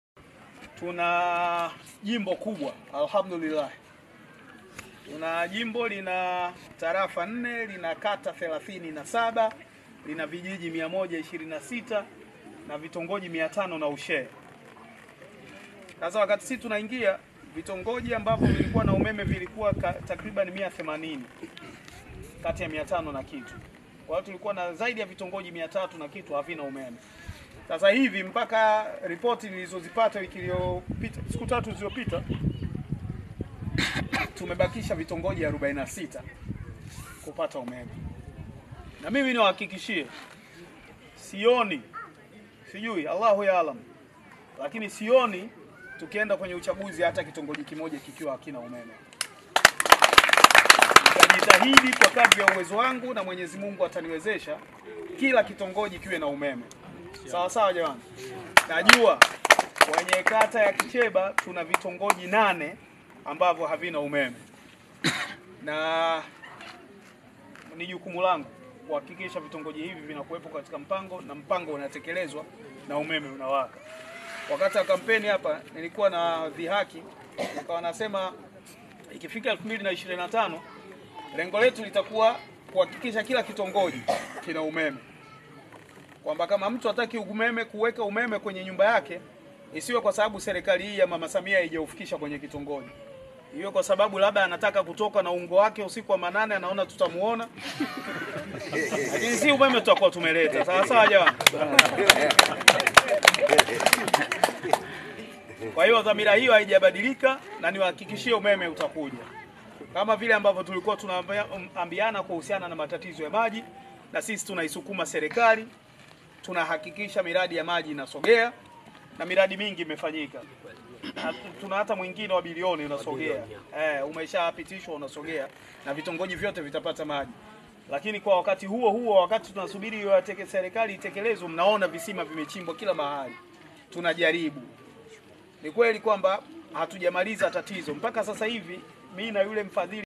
WANANCHI WA MUHEZA WAFURAHISHWA NA UTENDAJI KAZI WA MBUNGE WAO MWANA FA
Wananchi wa jimbo la Muheza  wanafurahishwa na utendaji kazi wa Mbunge wao Mhe. @MwanaFA ambaye sasa anafanya ziara Kijiji kwa Kijiji na kuwaelezea Wananchi Makubwa yanafanyika Chini ya Uongozi wa Rais Samia Jimboni Muheza..